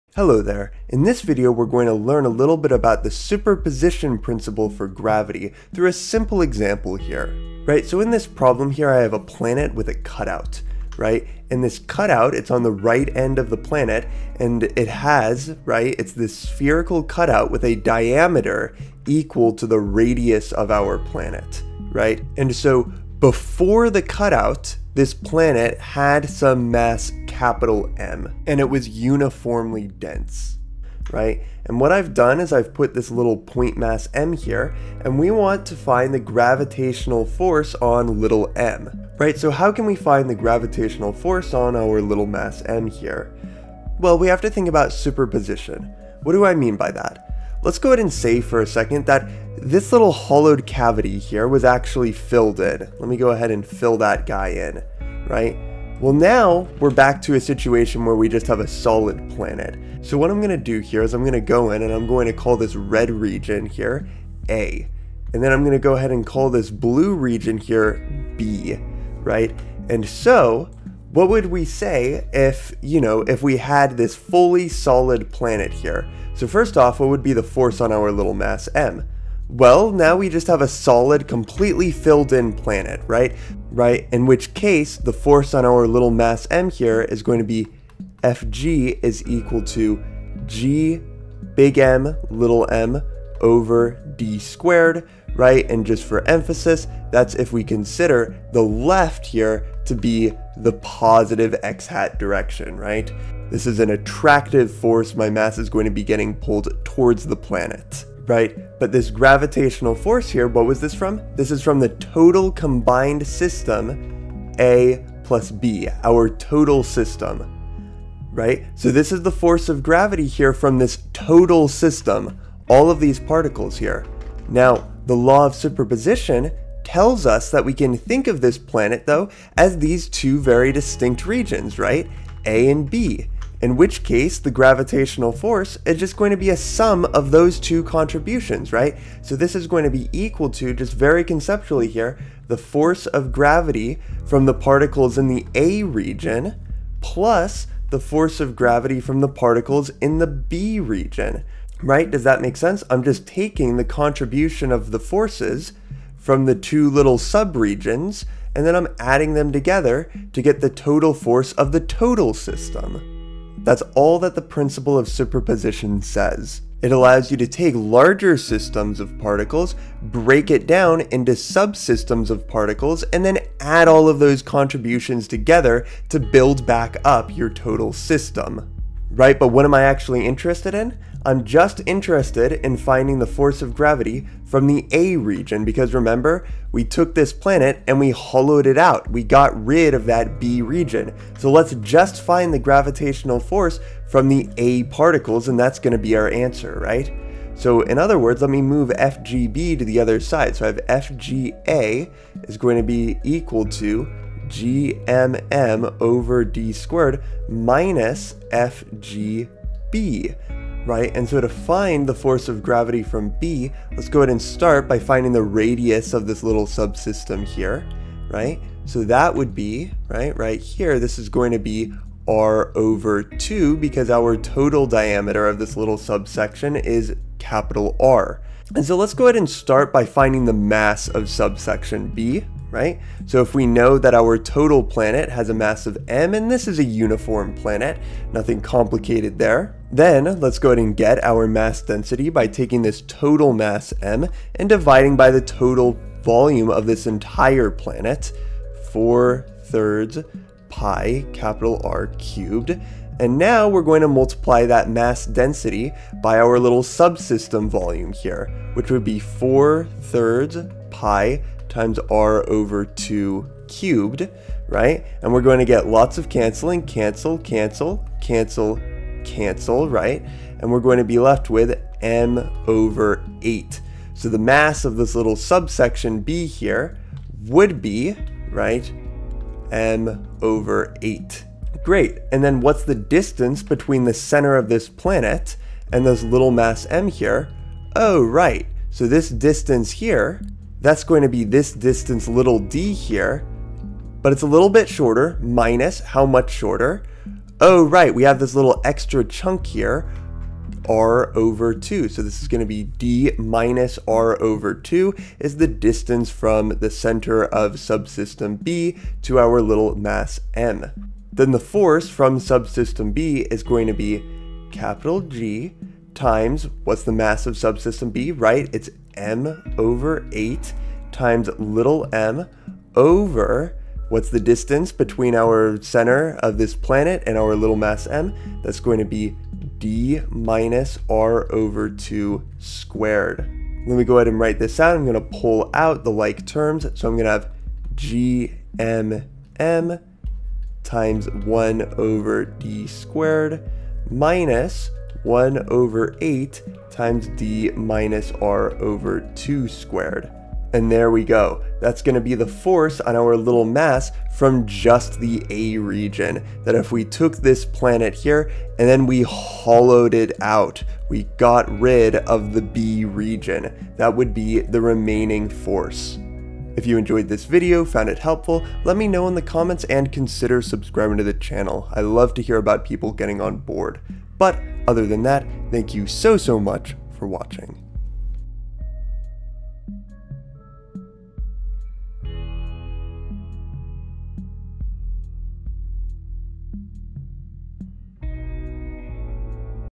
Superposition Principle for Gravity | A Simple Example
In this video we find the force of gravity on a particle from an asymmetric, hollowed out planet. Enjoy :3

Music!